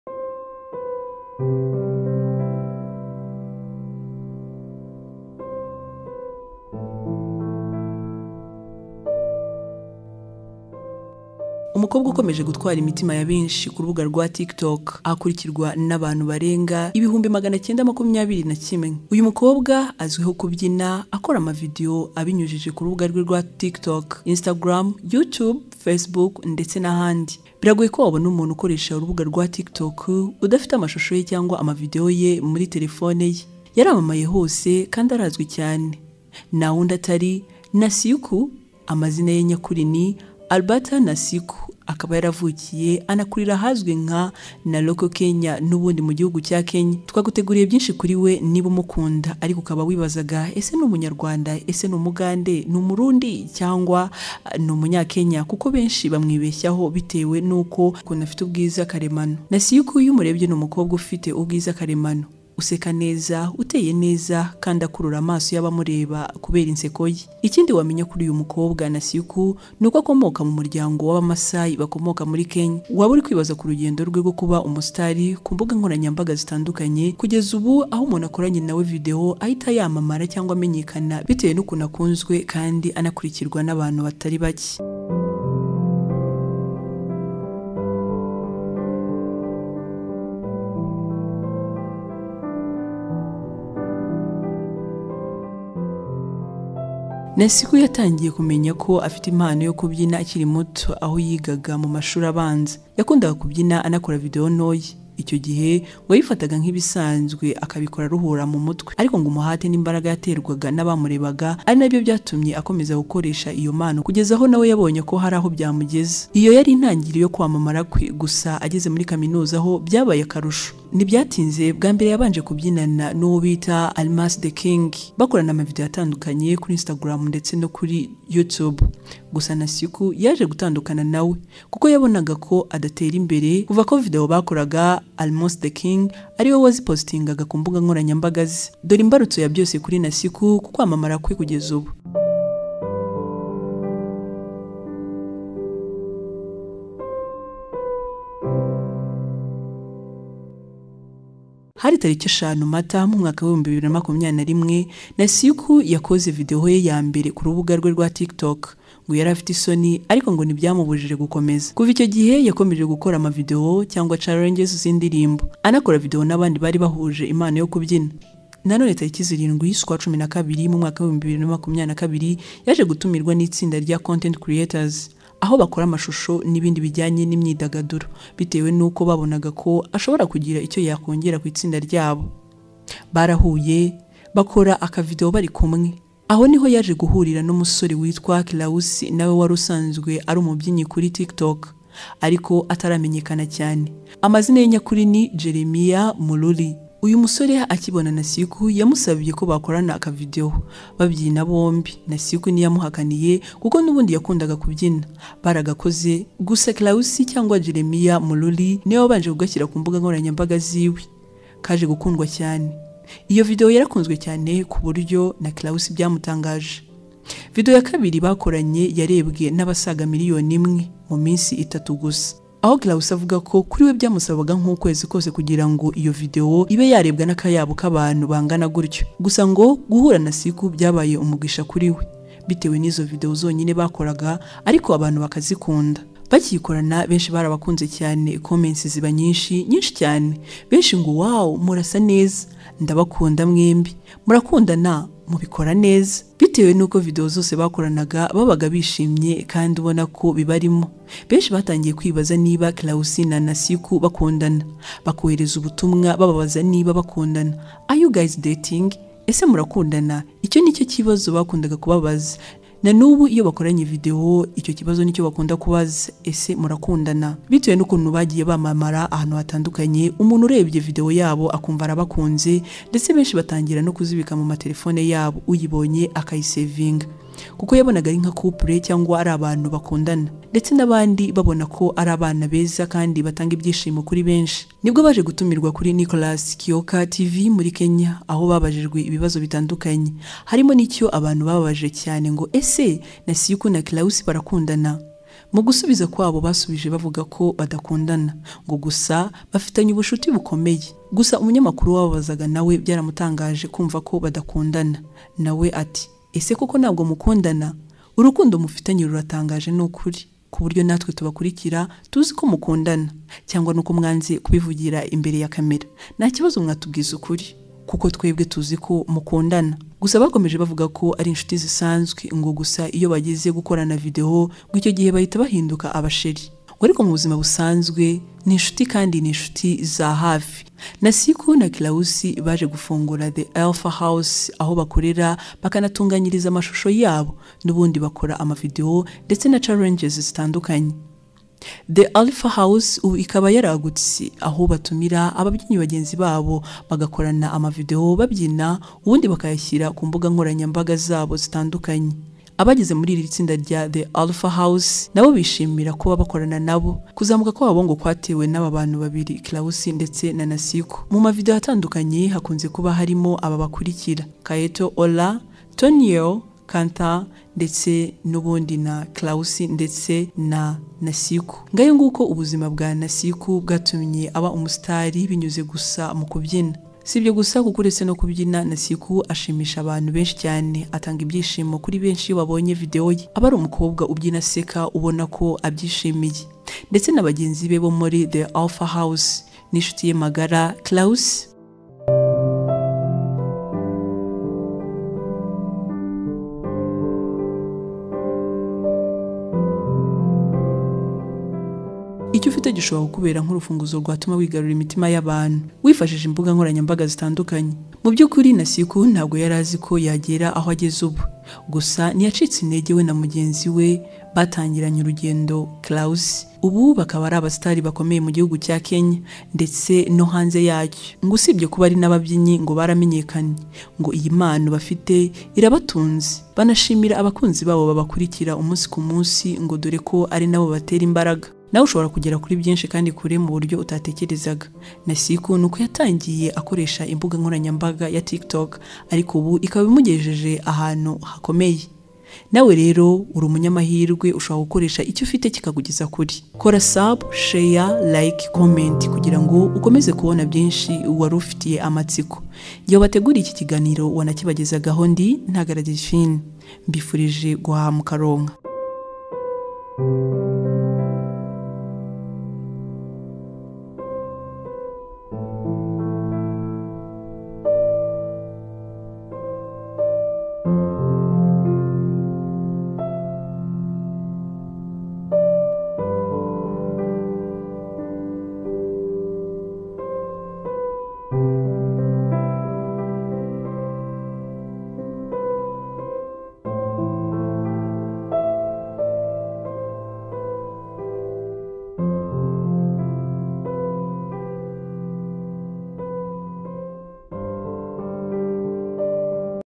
Nasieku TikToker: Menya byinshi ku mukobwa ukomeje gutwara imitima ya benshi kurubaga rwa TikTok.
Nasieku a TikToker from Kenya now she is vibrating allover the world with his friend Jerema.
comment, share , like, and sub to get know more❤️🥰